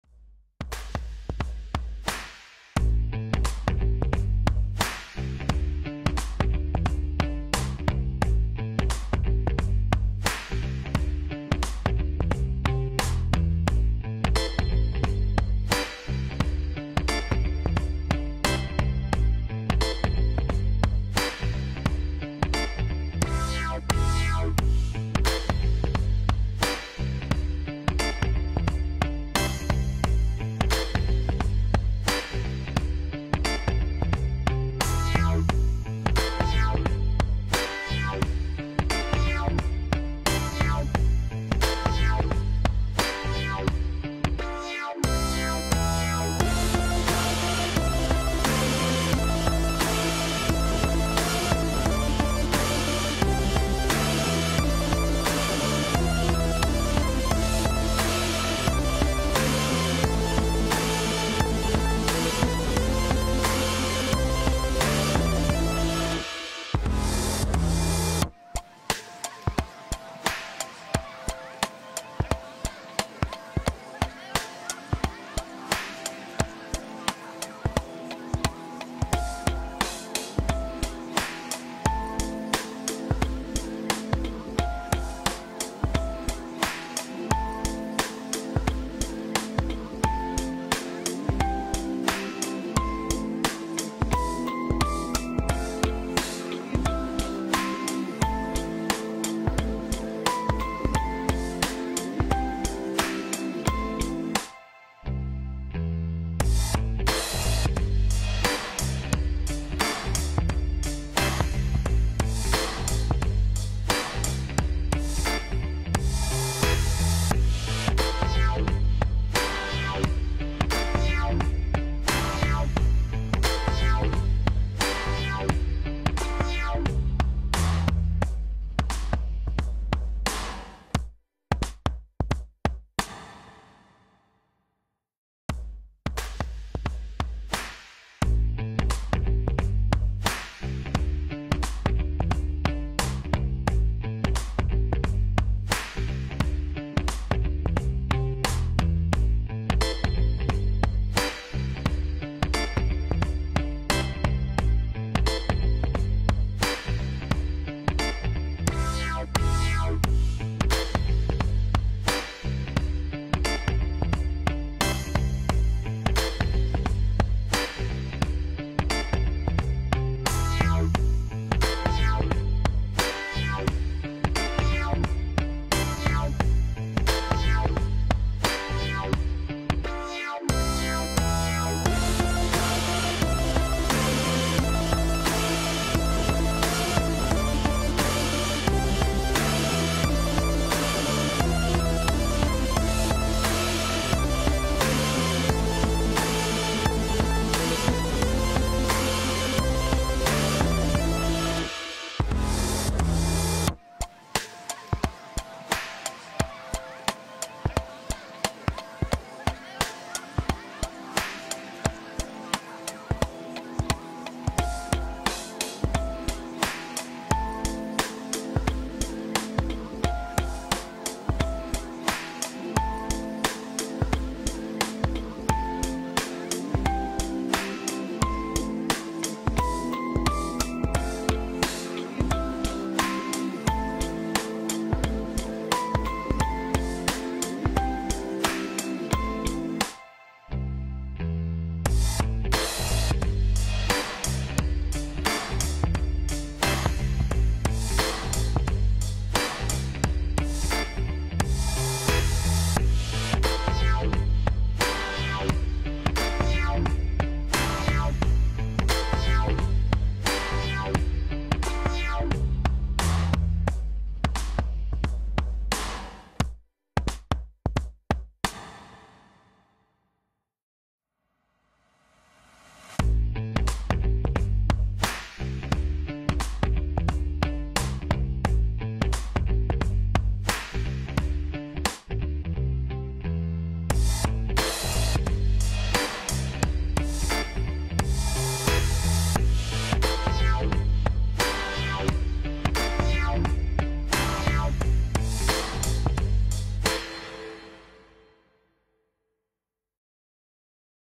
3D Print Hyperlapse | Ender 3 Pro 2020
Quick meaningful first test print on Ender 3 Pro | Cable Management Clip |